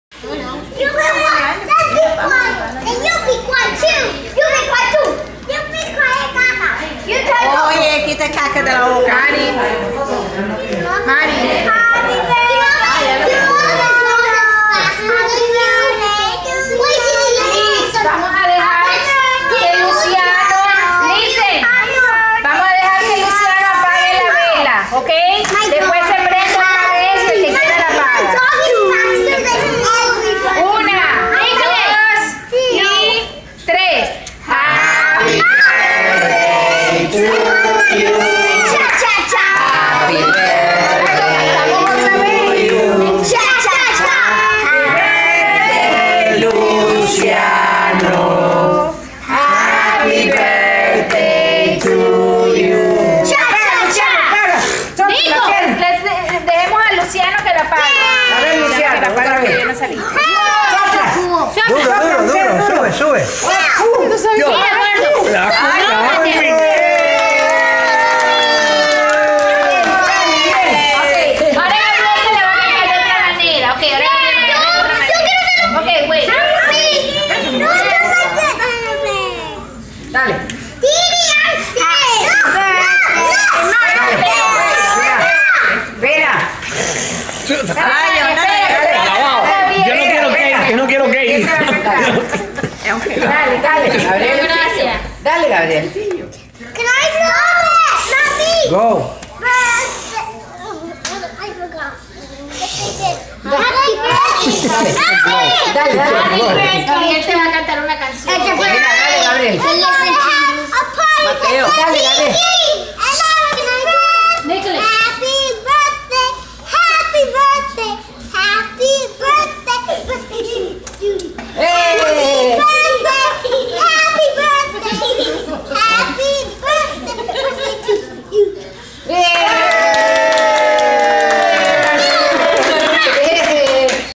Luciano's 3rd bday
Luciano having his wet cake